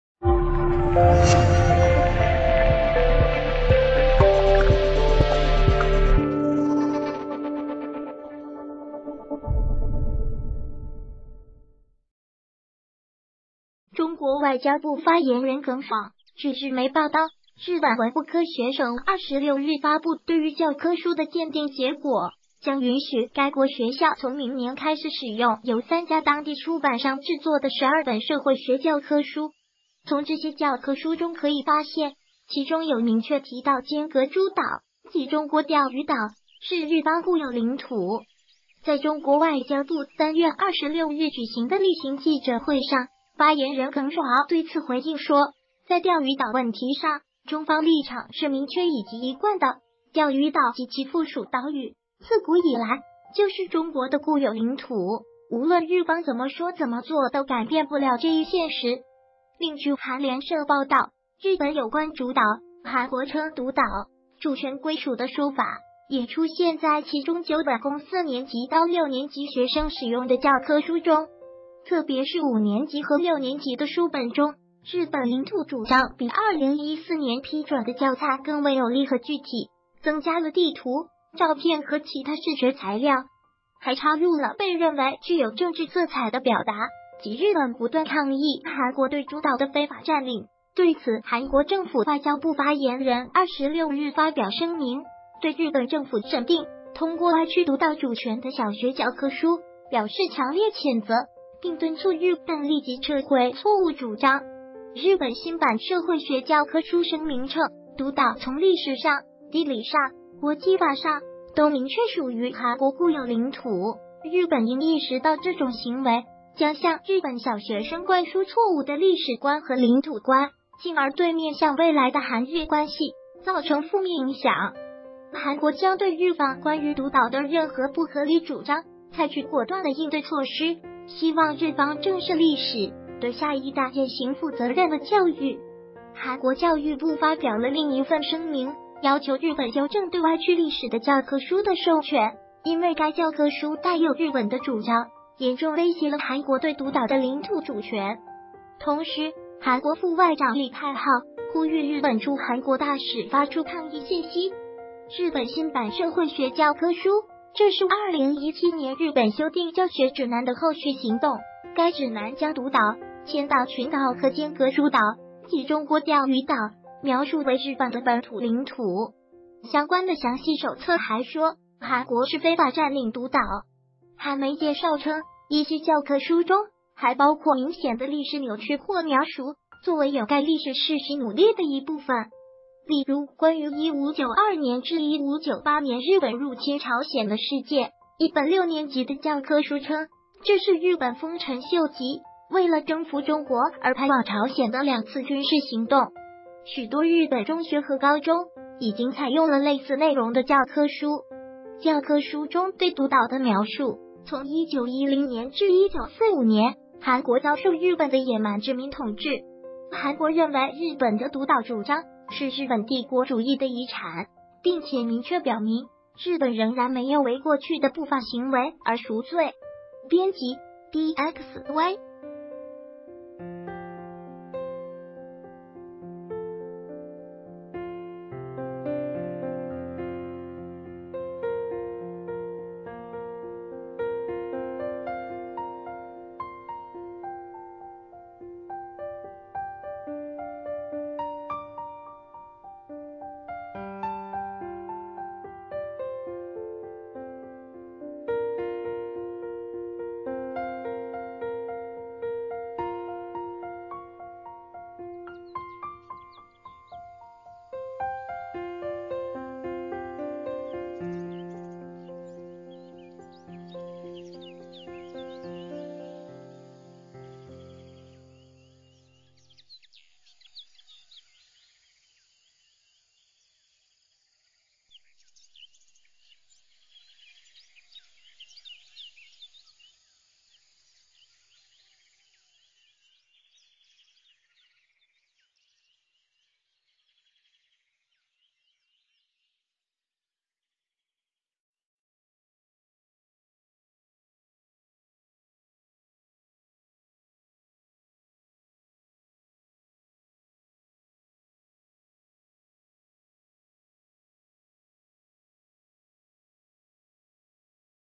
钓鱼岛写进日本教科书，外交部回应：改变不了固有现实
谢谢！中国外交部发言人耿爽据日媒报道，日本文部科学省26日发布对于教科书的鉴定结果，将允许该国学校从明年开始使用由三家当地出版商制作的12本社会学教科书。从这些教科书中可以发现，其中有明确提到“尖阁诸岛”（即中国钓鱼岛）是日方固有领土。在中国外交部3月26日举行的例行记者会上，发言人耿爽对此回应说，在钓鱼岛问题上，中方立场是明确以及一贯的，钓鱼岛及其附属岛屿自古以来就是中国的固有领土，无论日方怎么说怎么做都改变不了这一现实。另据韩联社报道，日本有关“竹岛”（韩国称独岛）主权归属的说法也出现在其中9本供四年级到六年级学生使用的教科书中。特别是五年级和六年级的书本中，日本领土主张比2014年批准的教材更为有力和具体，增加了地图、照片和其他视觉材料。还插入了被认为具有政治色彩的表达，即日本“不断抗议韩国对竹岛的非法占领”。对此韩国政府外交部发言人26日发表声明，对日本政府审定通过歪曲独岛主权的小学教科书表示强烈谴责，并敦促日本立即撤回错误主张。日本新版社会学教科书声明称，独岛从历史上、地理上、国际法上都明确属于韩国固有领土，日本应意识到这种行为将向日本小学生灌输错误的历史观和领土观，进而对面向未来的韩日关系造成负面影响。韩国将对日方关于独岛的任何不合理主张采取果断的应对措施，希望日方正视历史，对下一代践行负责任的教育。韩国教育部发表了另一份声明，要求日本纠正对歪曲历史的教科书的授权，因为该教科书带有日本的主张，严重威胁了韩国对独岛的领土主权。同时，韩国副外长李泰昊呼吁日本驻韩国大使发出抗议信息。日本新版社会学教科书这是2017年日本修订教学指南的后续行动，该指南将独岛、千岛群岛和“尖阁诸岛”(即中国钓鱼岛)描述为日本的“本土”领土。相关的详细手册还说，韩国是“非法占领”独岛。韩媒介绍称，一些教科书中还包括明显的历史扭曲或描述，作为掩盖历史事实努力的一部分。例如关于1592年至1598年日本入侵朝鲜的事件，一本六年级的教科书称，这是日本丰臣秀吉为了征服中国而派往朝鲜的两次军事行动。许多日本中学和高中已经采用了类似内容的教科书。教科书中对独岛的描述从1910年至1945年，韩国遭受日本的野蛮殖民统治。韩国认为日本的独岛主张是日本帝国主义的遗产，并且明确表明日本仍然没有为过去的不法行为而赎罪。（编辑：DXY）